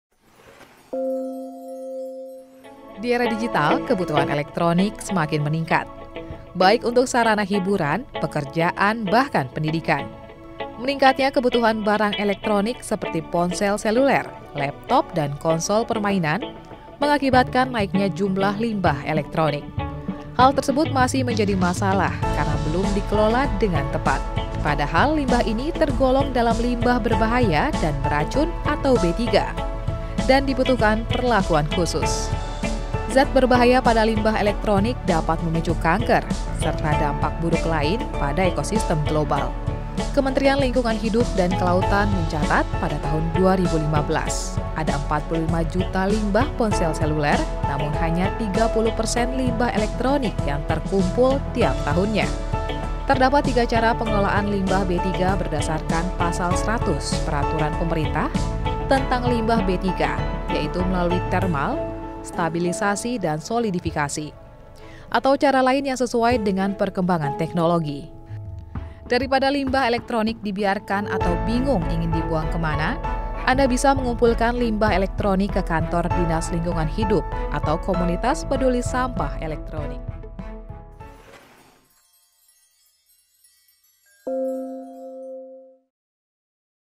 Limbah Elektronik
Di Indonesia sendiri terdapat jutaan limbah elektronik yang berbahaya. Kandungan zat kimia yang terkandung pada komponen limbah elektronik bisa membahayakan kesehatan.

Oleh sebab itu, mari bersama kita bijak dalam penggunaan elektronik sampai dengan proses pengolahan limbahnya untuk keselamatan banyak orang dan lingkungan.